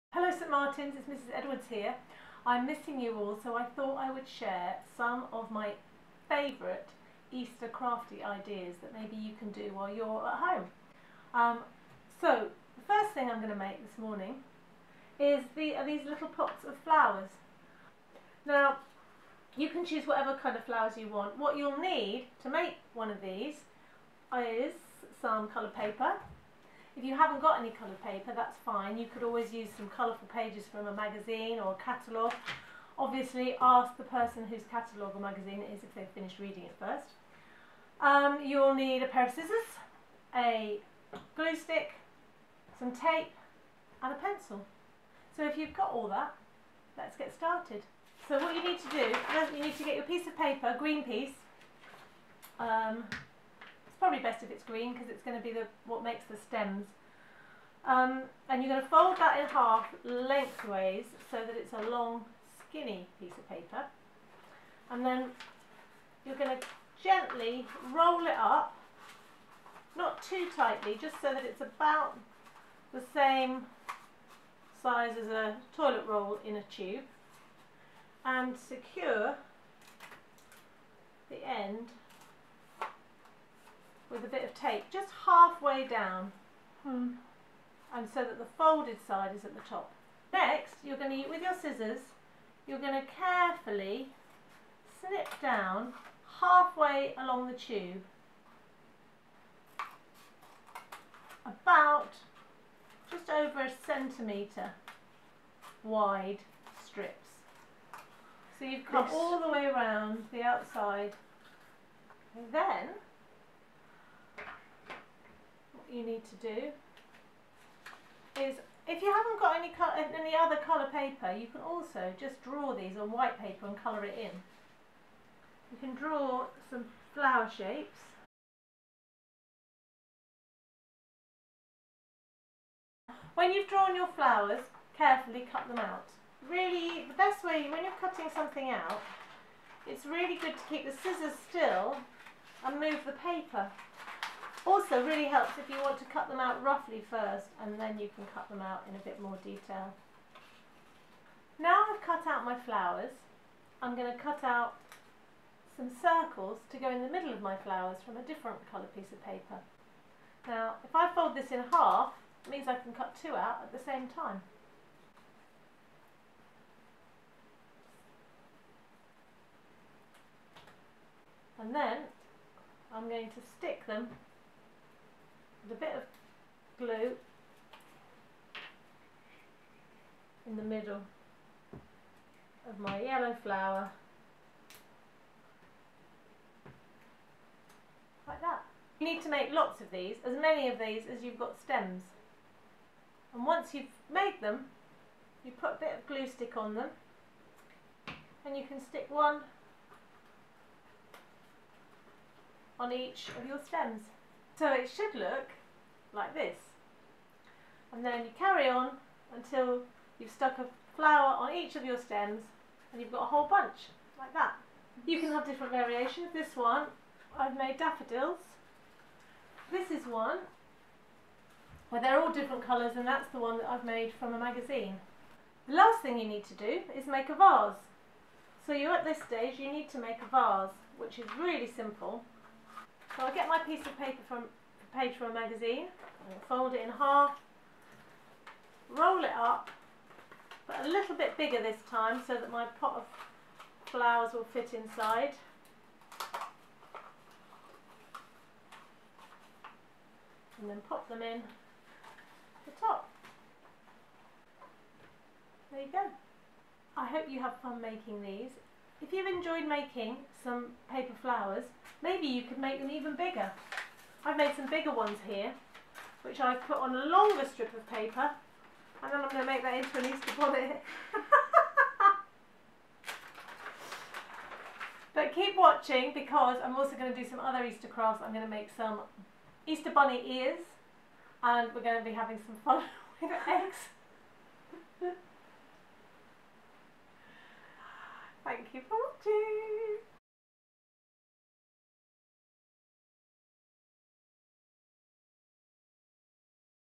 Easter crafts with Mrs Edwards
Mrs Edwards - our favourite crafty lady - gives us some great ideas for Easter crafts!